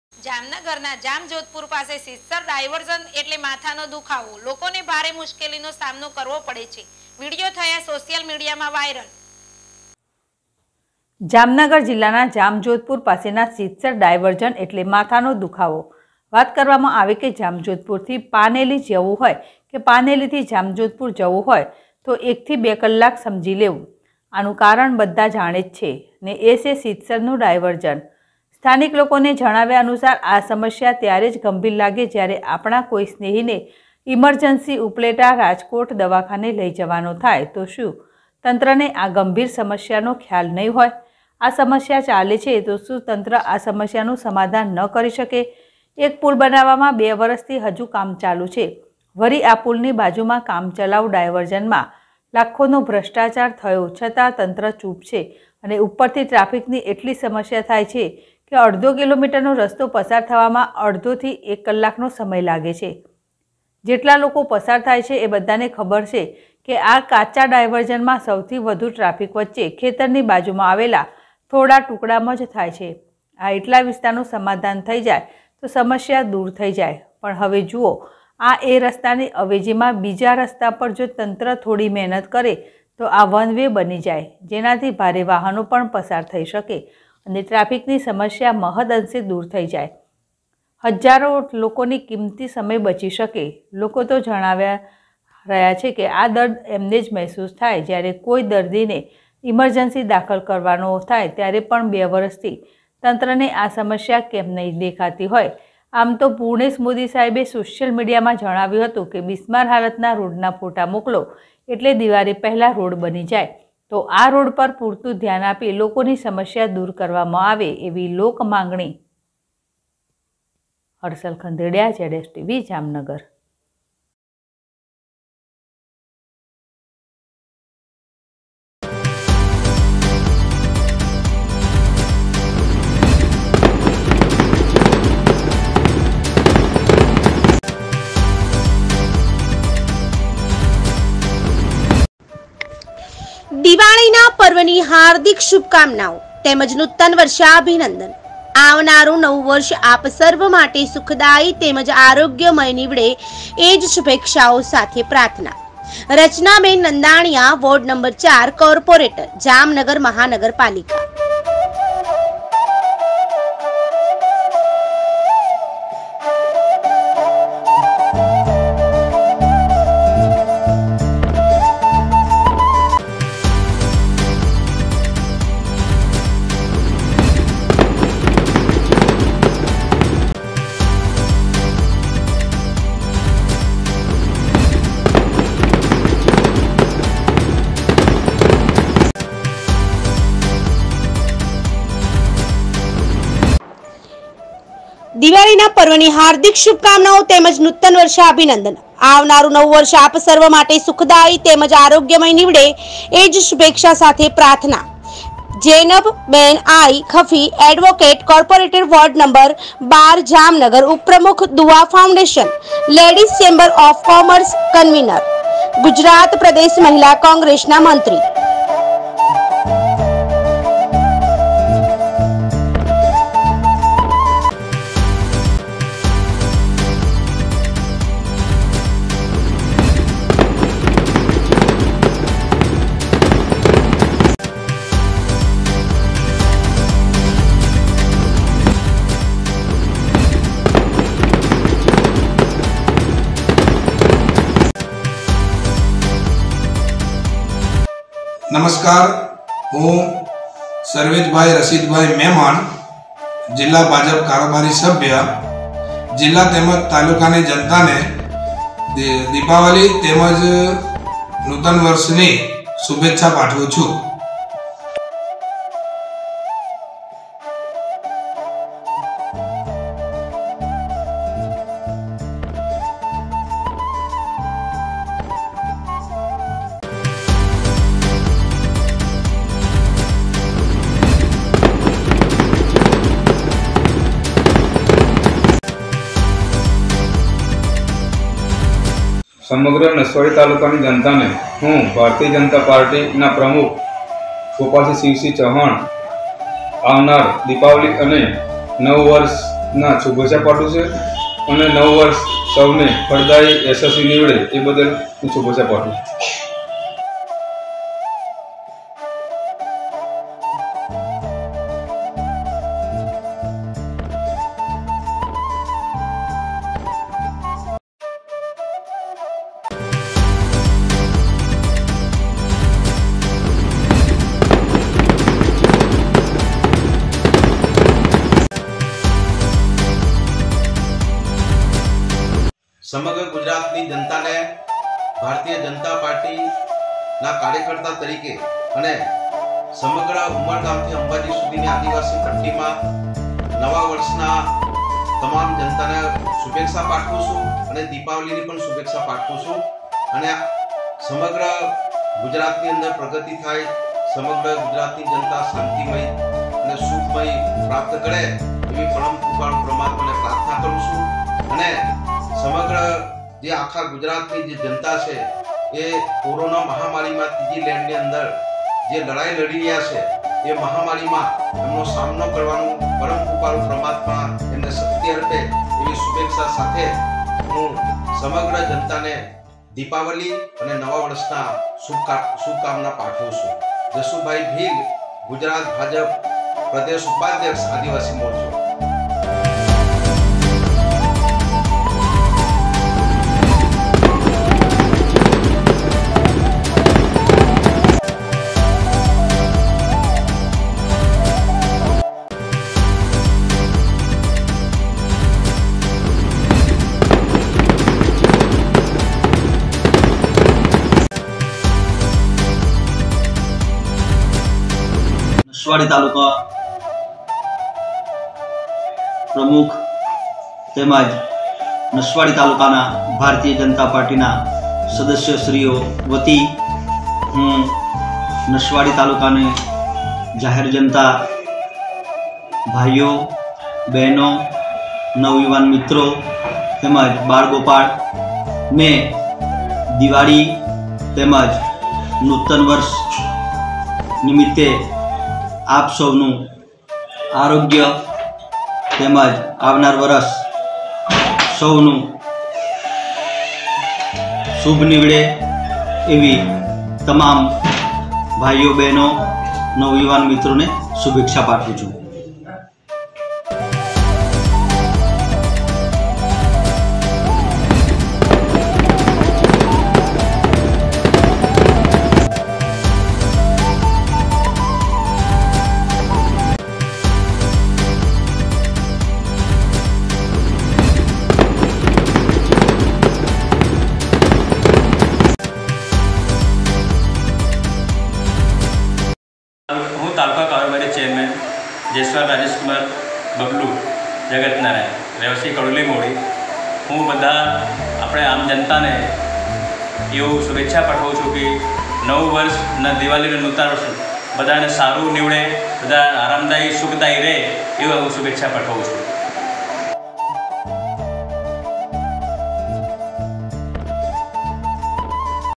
જામજોધપુર પાસે ના સિદસર ના ડાઇવર્ઝન એટલે માથાનો દુખાવો. સોસિયલ મિડિયા માં વિડિયો વાયરલ ..
જામજોધપુર પાસે ના સિદસર ના ડાઇવર્ઝન એટલે માથાનો દુખાવો. સોસિયલ મિડિયા માં વિડિયો વાયરલ ..// ZSTVNEWS/